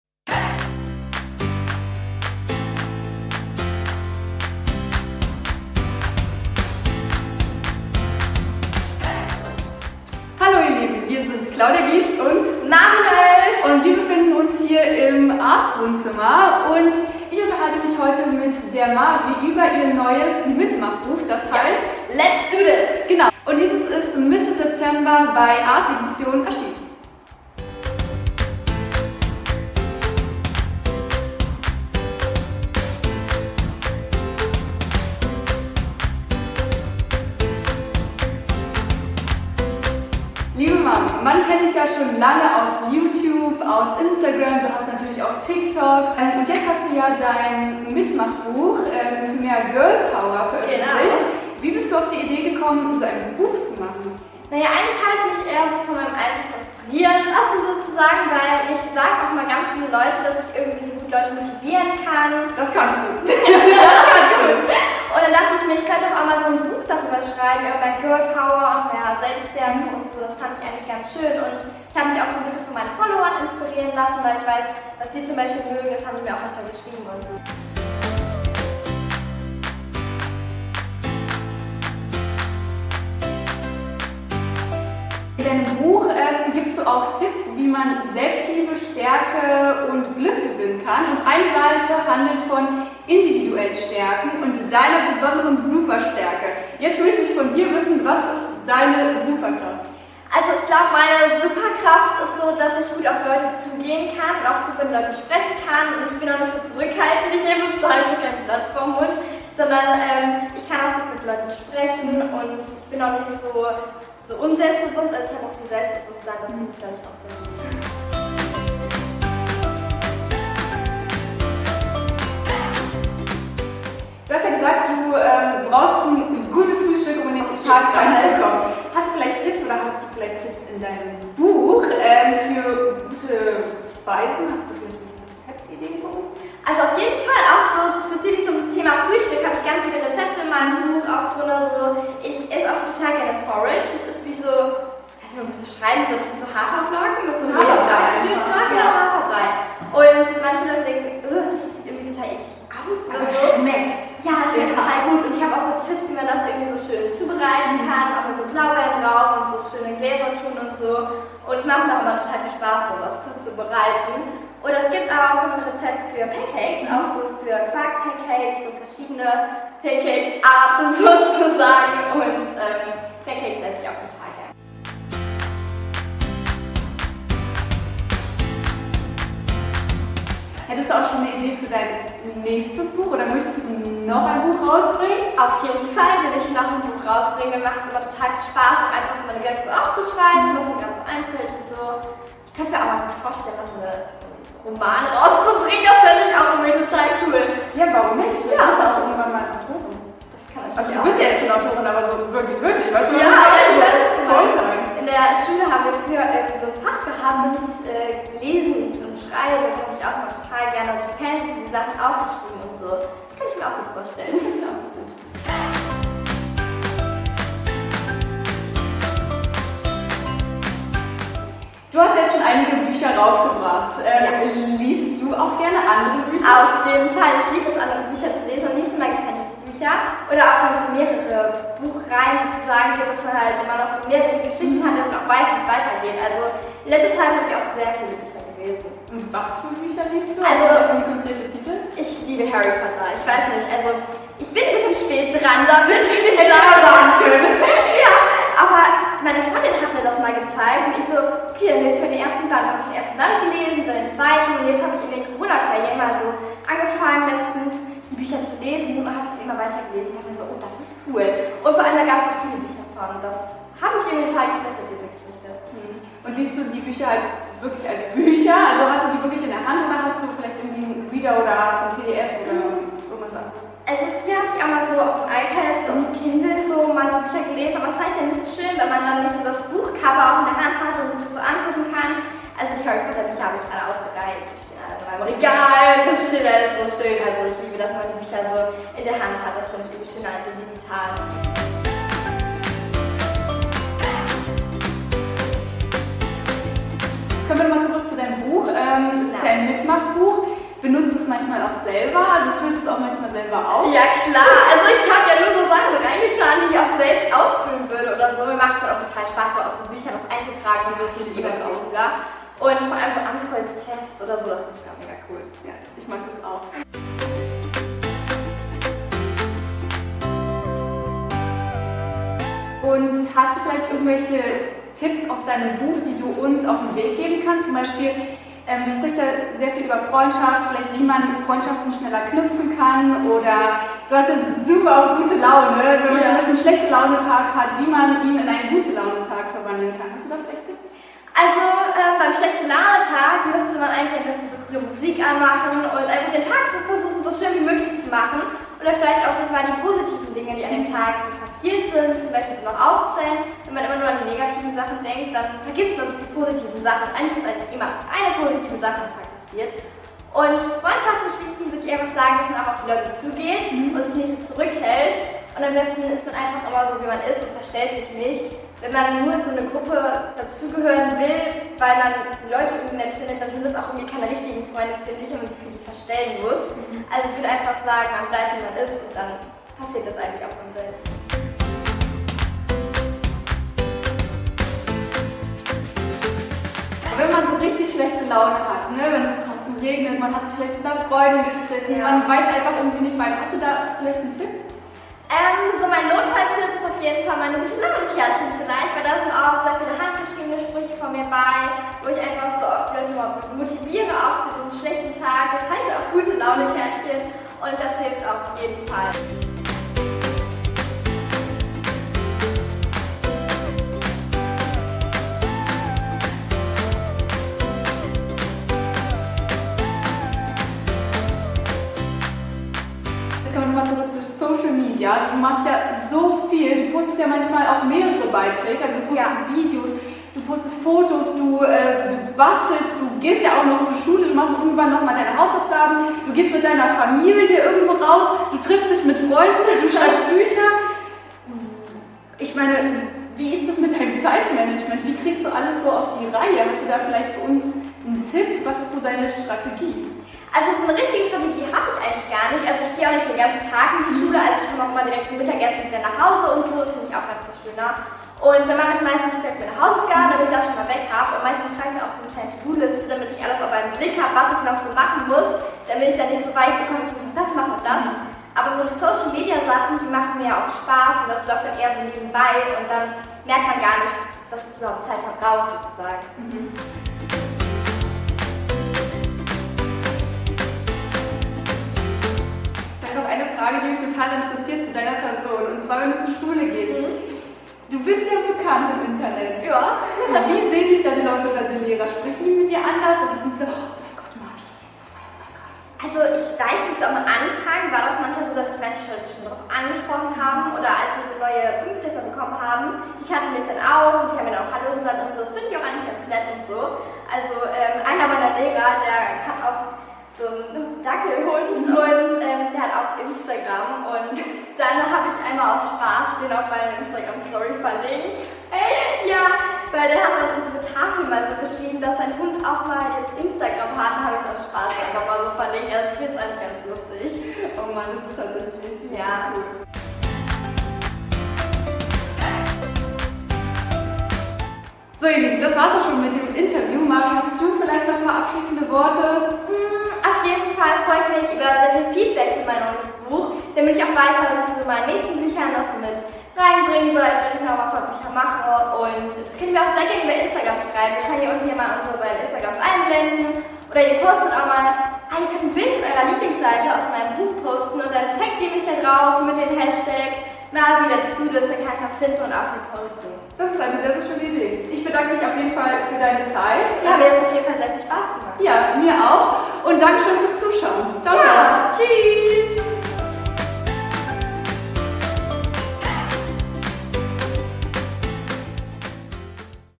Erzähl doch mal: Mavie Noelle im Gespräch mit Klaudia mit K
"Let's do this!" lautet der Titel des neuen Buches von YouTuberin Mavie Noelle und der Name ist Programm: die erfolgreiche Influencerin schreibt über bunte Ideen für den Alltag, mehr Motivation und Glück - und über ganz viel Girl Power.
GNTM-Star "Klaudia mit K" spricht mit Mavie übers Schreiben, Positivität und Notfallrezepte gegen schlechte Laune.